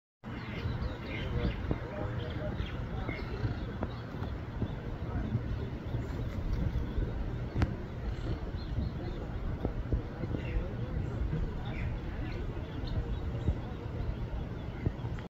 Confusion of keets
Baby Guinea fowl, called keets, learning the ropes from the adult hens.